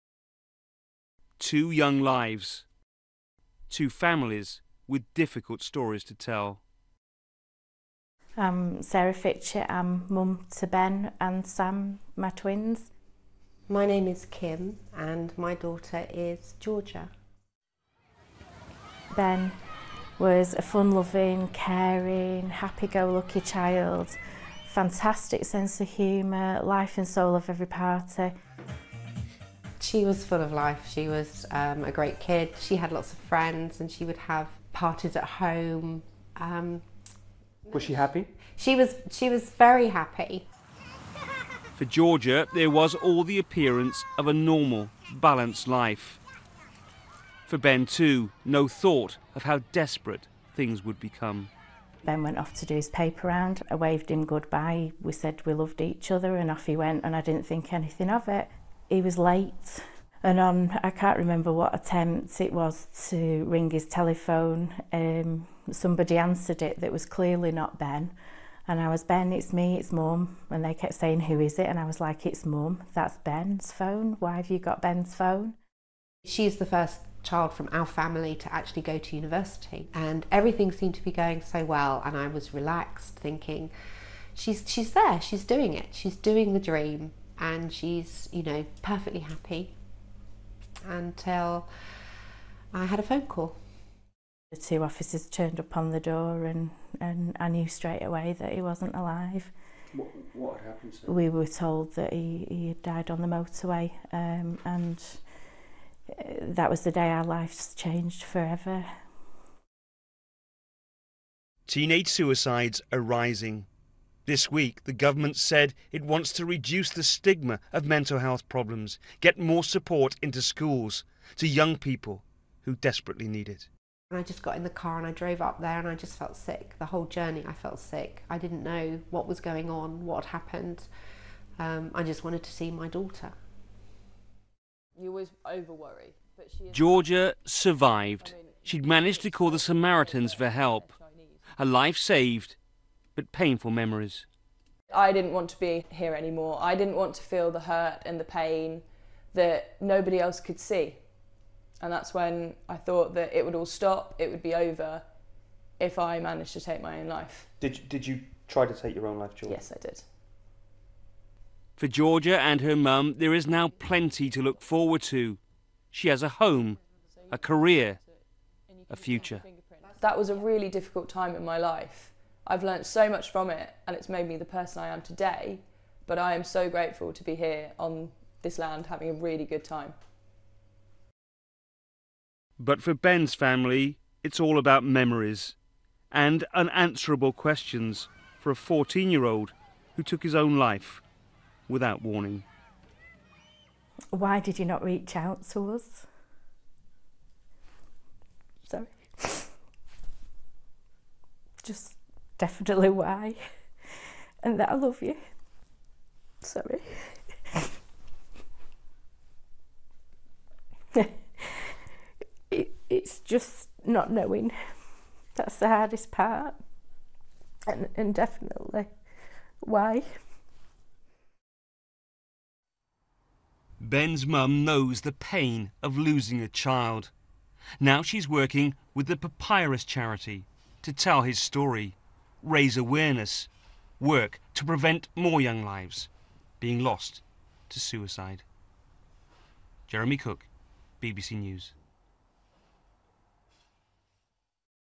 Teenage suicide: Two mothers tell their children's stories
PAPYRUS Trustee Sarah Fitchett speaks to the BBC about losing her son Ben to suicide.